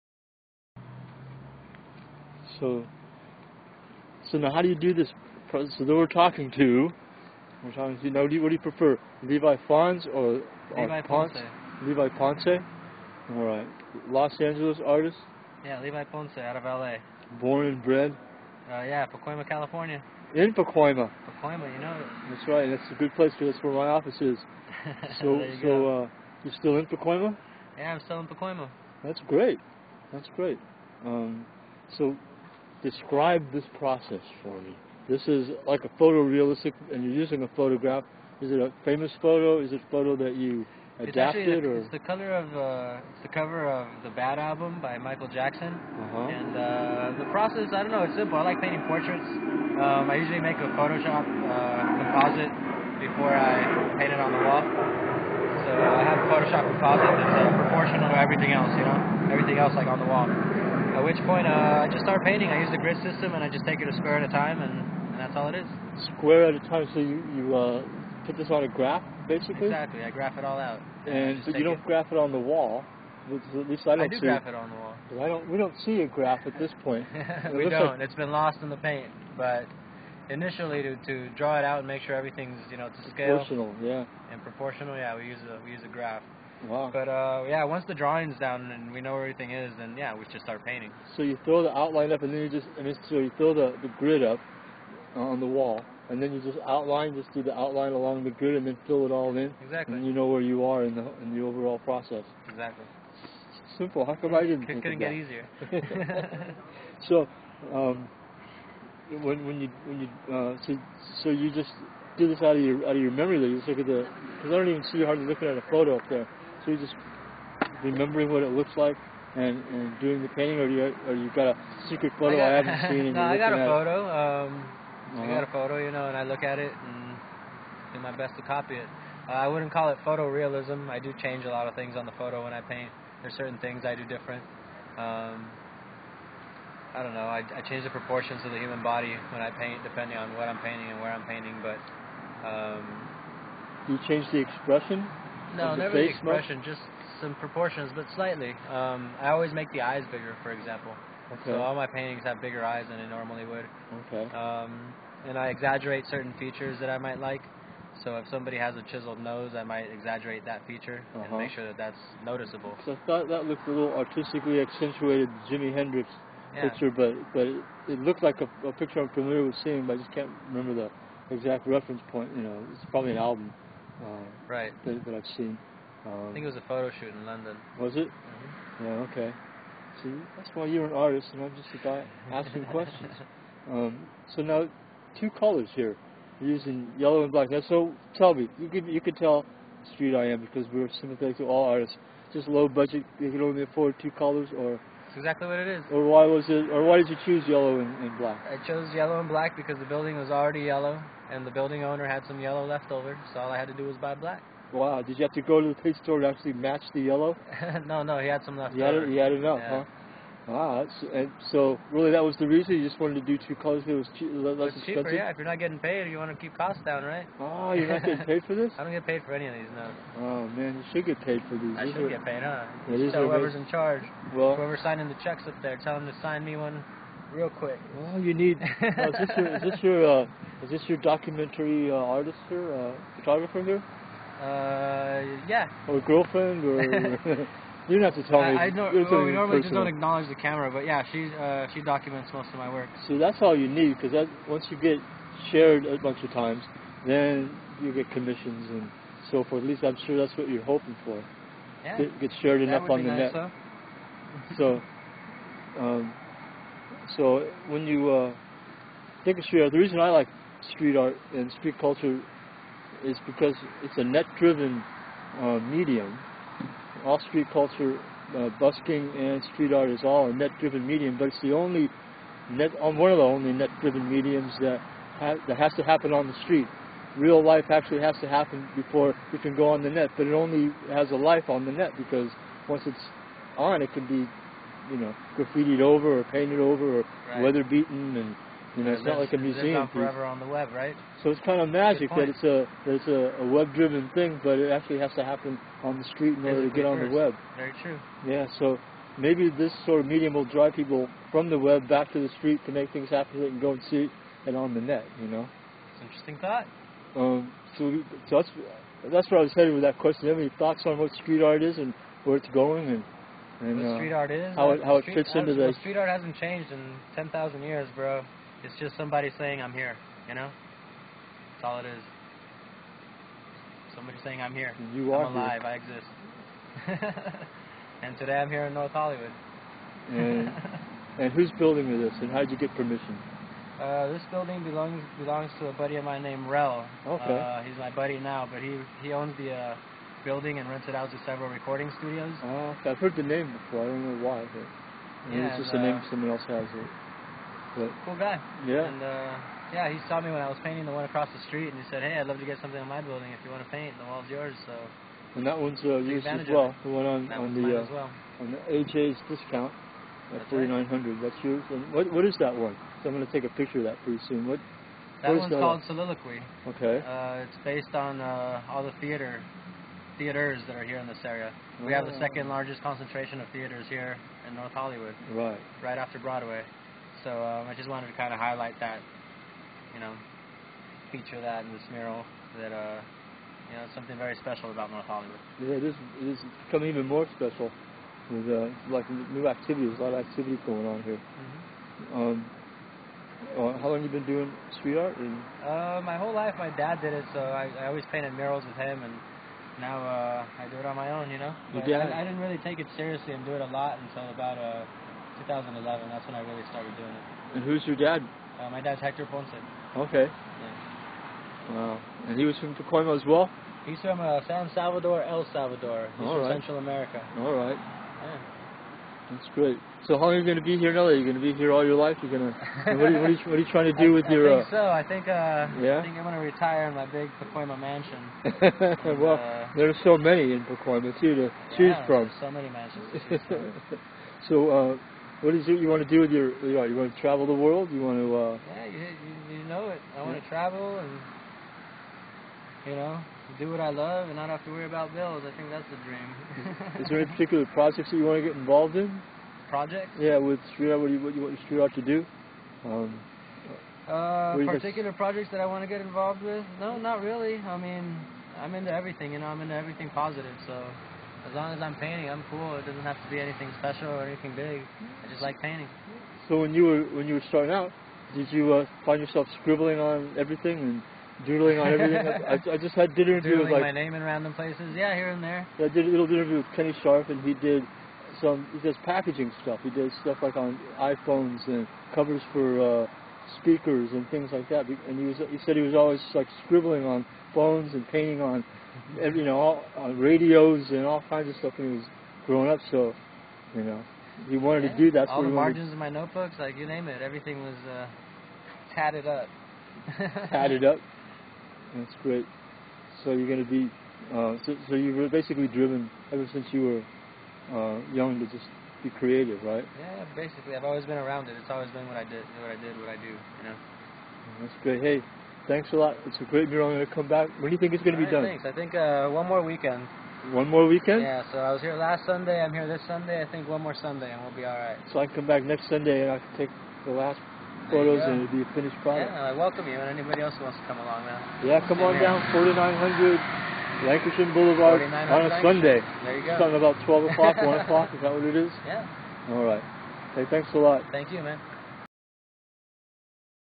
LA STREET ARTIST LEVI PONCE INTERVIEW, Street I Am TV recorded live on 8/10/14 at 3:28 PM PDT
He is painting 2 Amazing art murals. One wall with 2 larger than life pop stars painted on it. We talked to the artist, Levi Ponce on his break and got a little insight into the world of a Pacoima street art legend from a street art family.
Recorded live from United States, California, North Hollywood on my iPhone on 8/10/14 at 3:28 PM PDT - Captured Live on Ustream